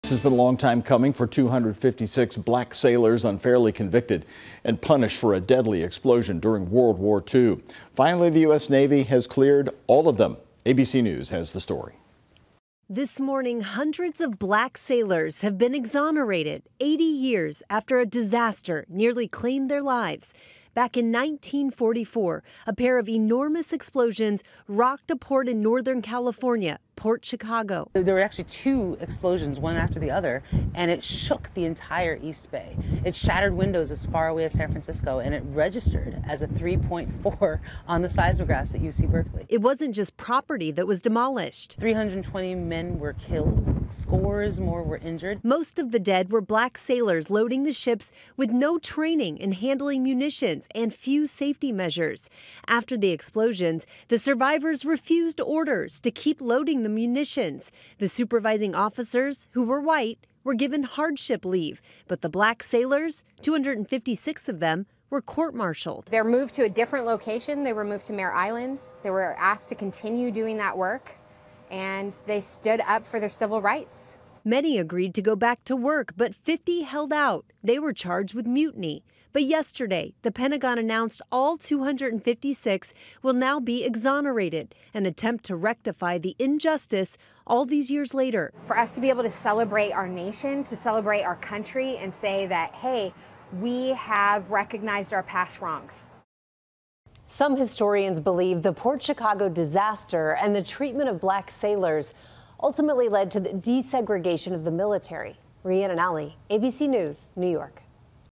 Hundreds of Black sailors exonerated 80 years after California explosions
Some historians believe the port-Chicago disaster, and the treatment of the Black sailors, led to the desegregation of the military.